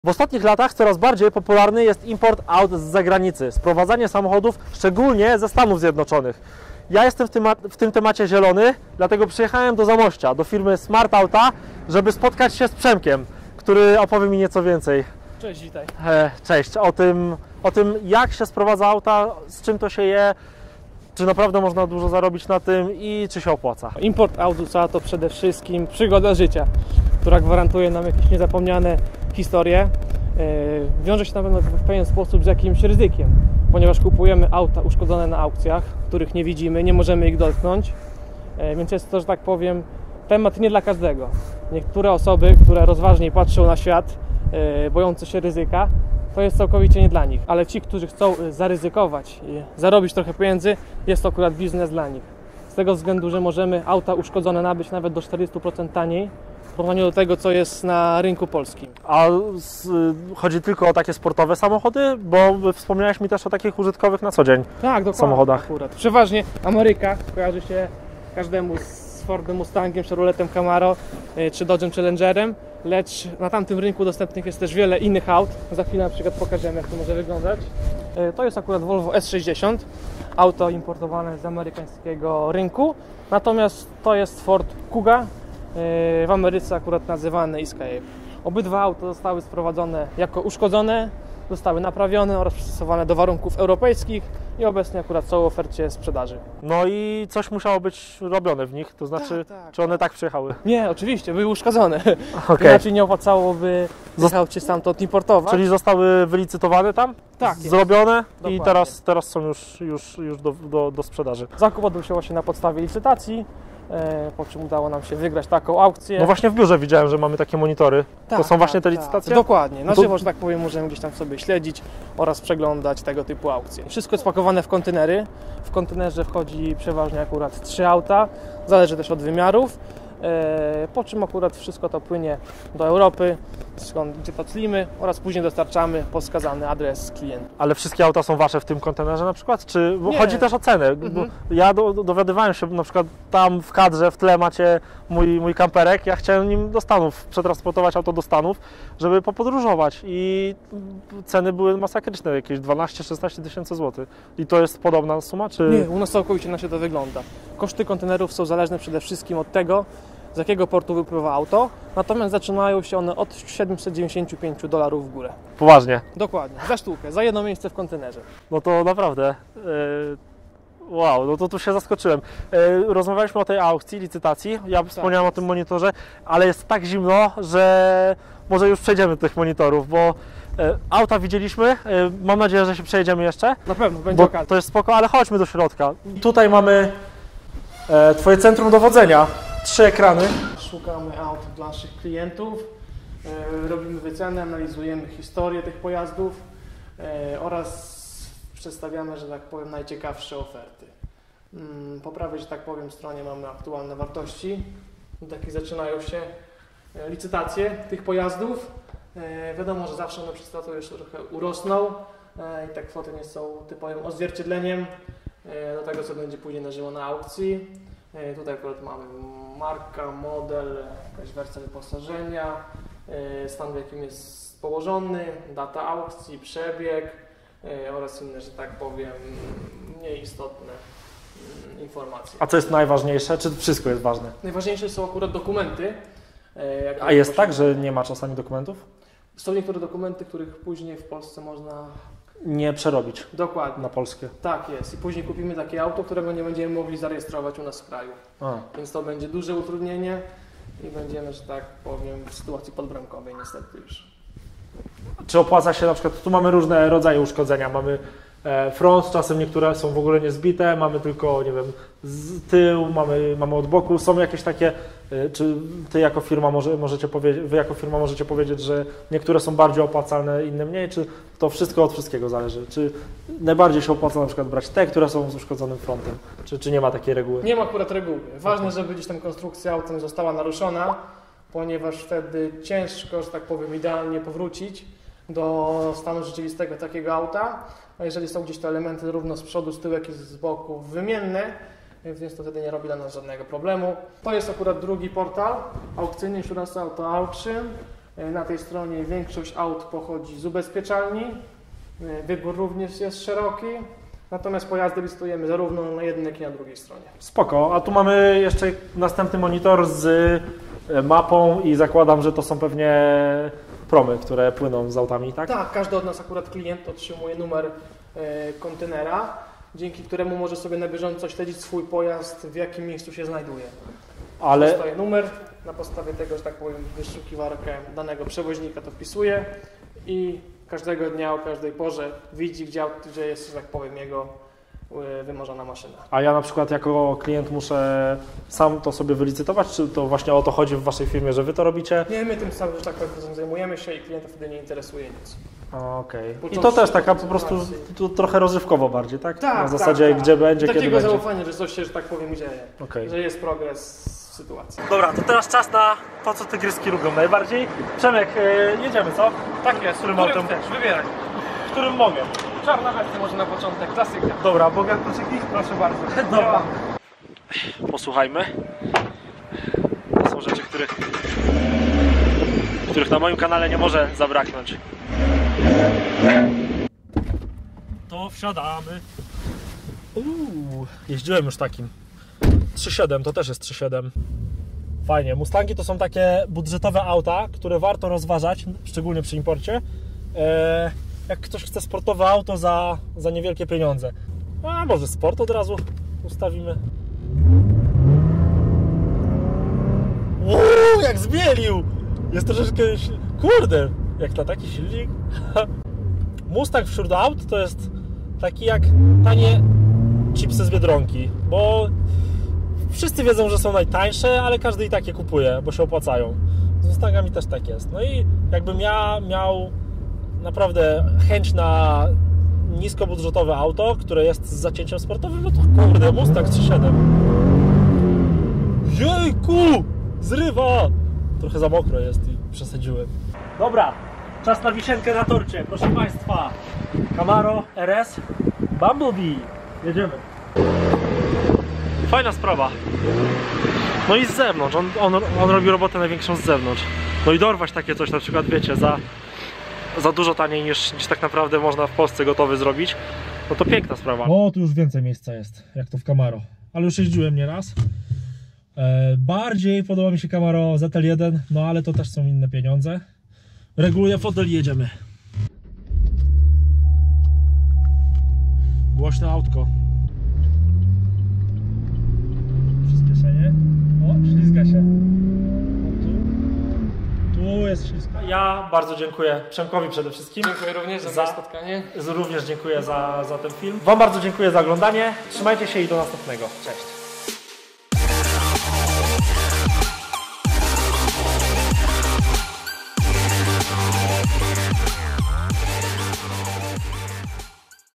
Auta z USA - jak na tym zarobić:
Widzicie właśnie krótki film o imporcie aut z USA.
🔸Jak wygląda proces sprowadzenia aut?
🔸Jak kupować auta, żeby na tym zarobić?
🔸Które auta najlepiej się sprzedają?

Na te pytania znajdziecie odpowiedź w dzisiejszym materiale. Motoryzacja Europejska idzie w bardziej ekologicznym, unormowanym kierunku i wiele osób marzy o prawdziwym potężnym amerykańskim aucie, z wielkim silnikiem i ogromną mocą. Import aut z USA to chyba najlepsze rozwiązanie, szczególnie jeśli szukamy samochodu używanego.